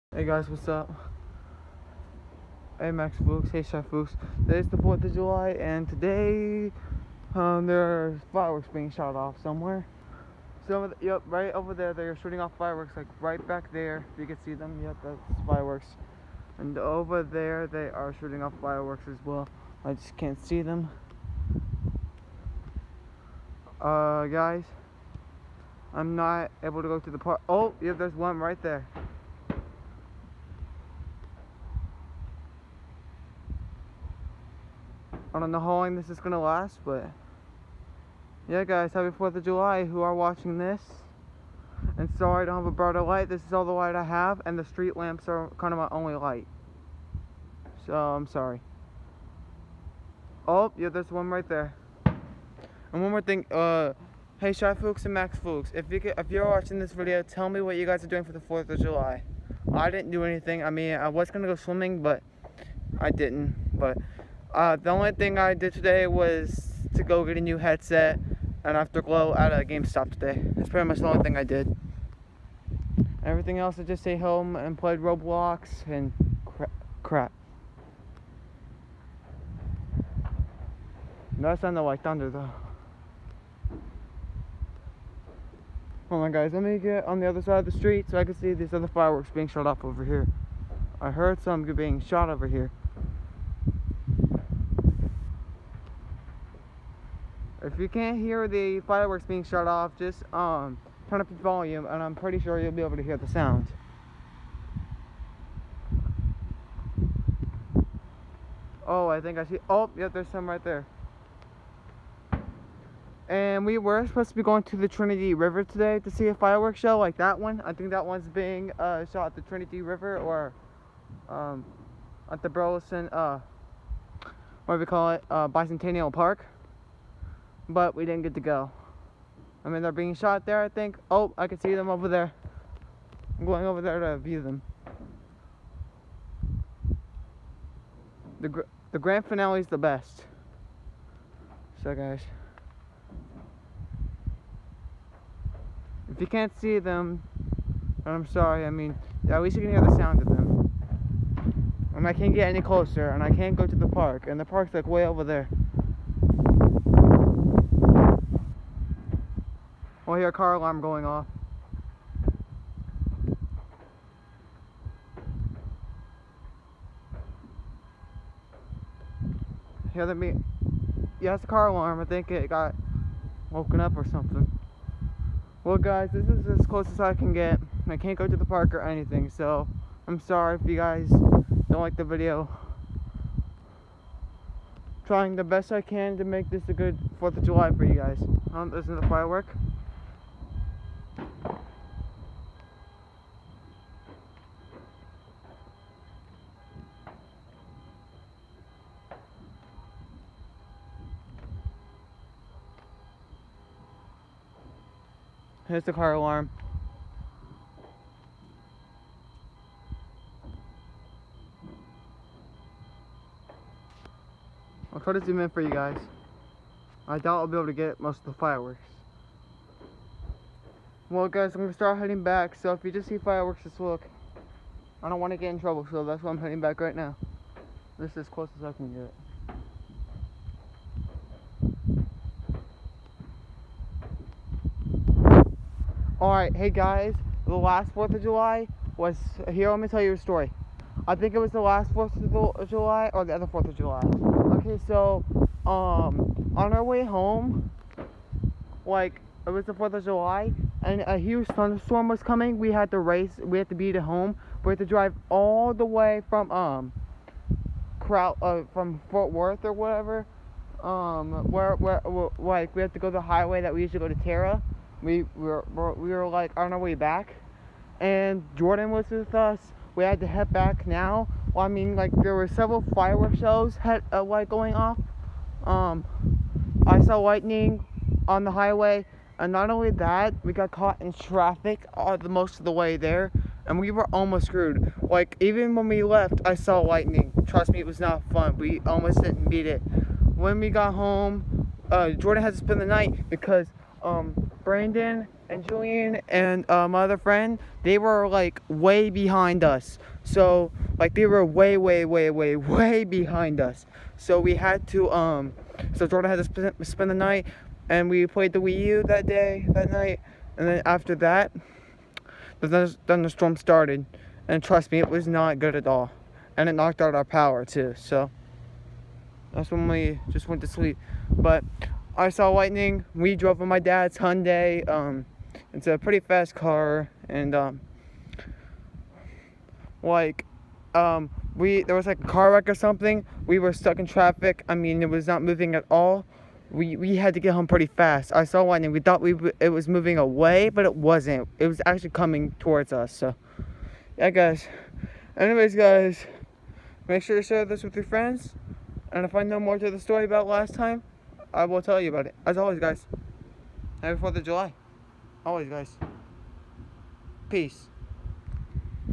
The forth of July
Watching a firework show for you max foox and shy foox